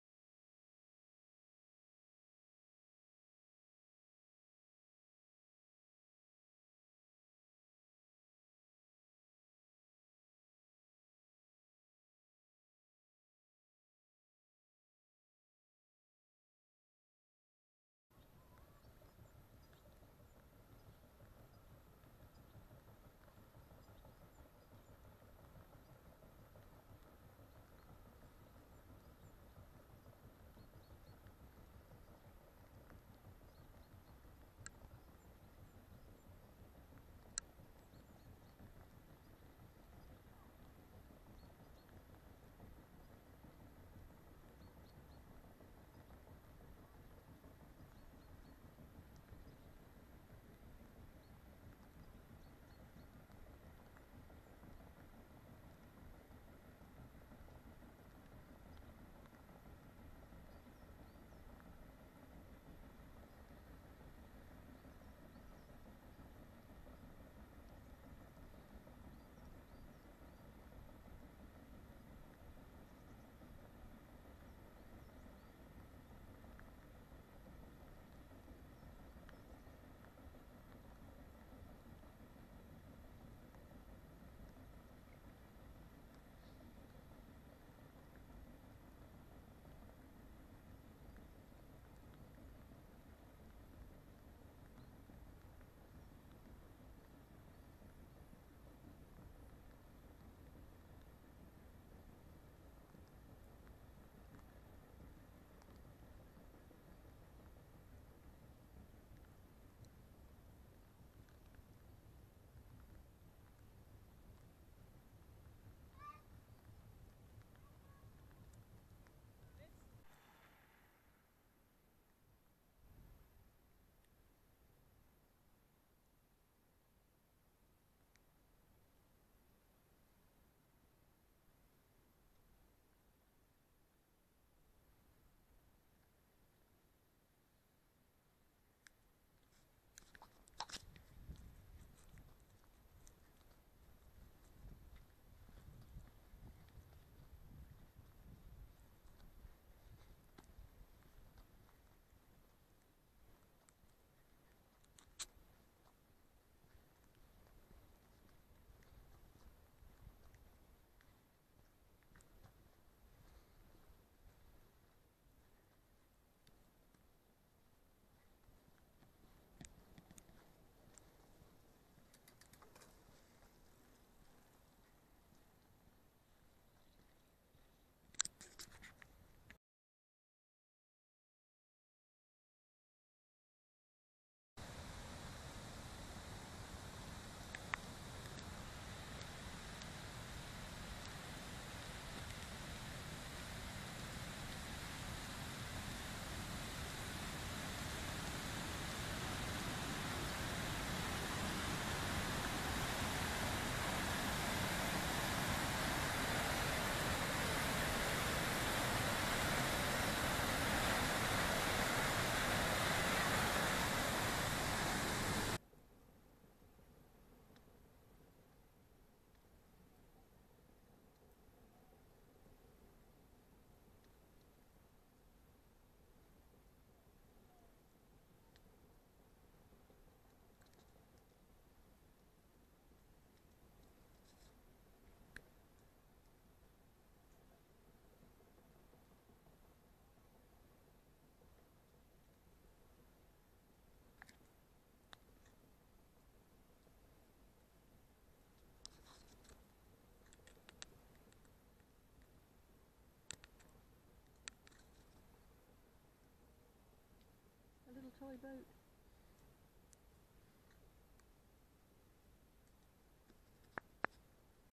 Walking Tignabruiach with paddle steamer Waverley sail-by
This video is of a walk from Tignabruiach to Glen Caladh.  During the walk the paddle steamer Waverley arrived and departed.  The village hugs the shore and the misty conditions lent an etherial quality to the views.
Nothing quite like the thump thump of paddles echoing in the hills. Otherwise it was gloriously quiet until we came to a rushing torrent cascading down toward the West Glen. We spotted an outlook point near Glen Caladh to watch the Waverley approach the Kyles of Bute.